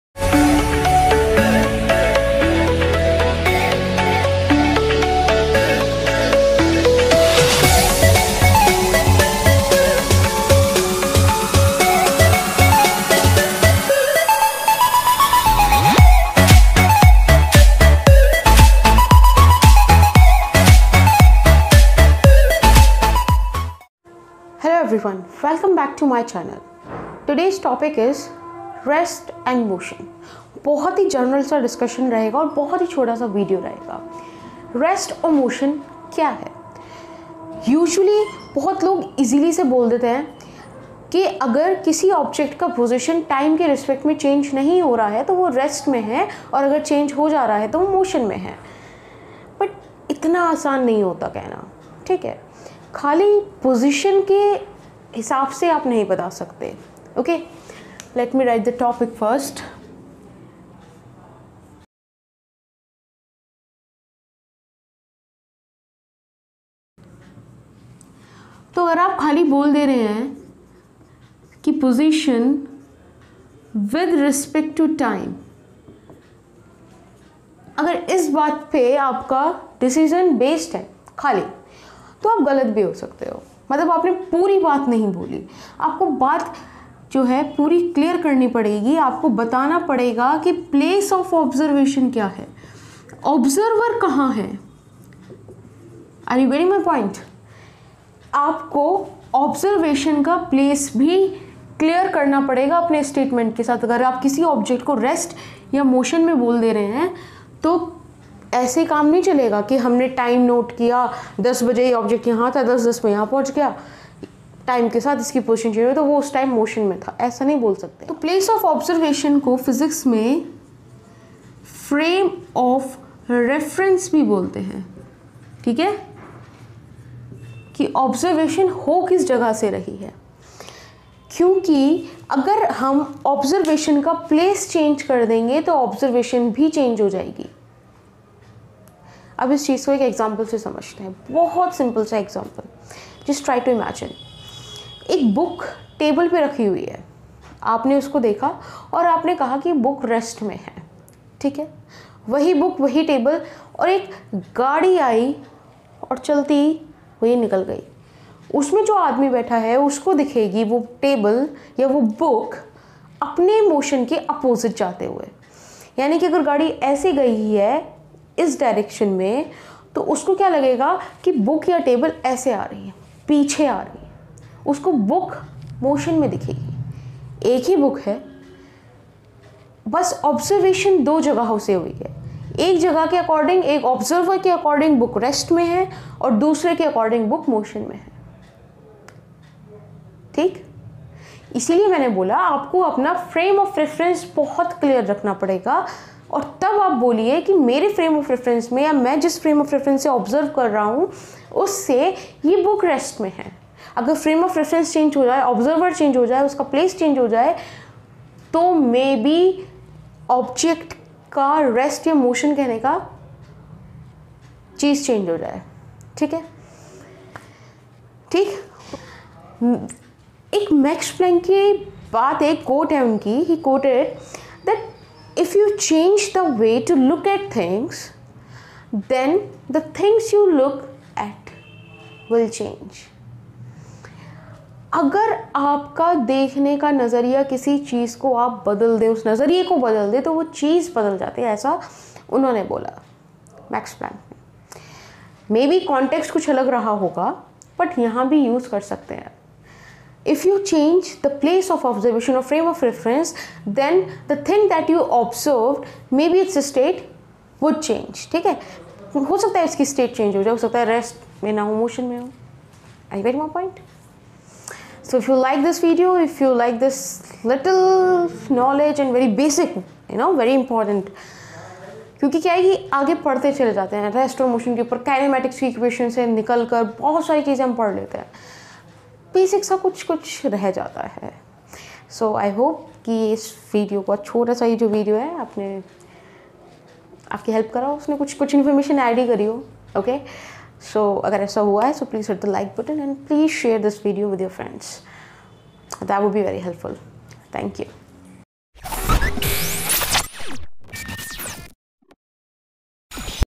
Rest and Motion | Basics | Mechanics | General discussion | NCERT | B.Sc
In this video, I introduced the concept of frame of reference and in further few videos I will discuss it( frame of reference) further.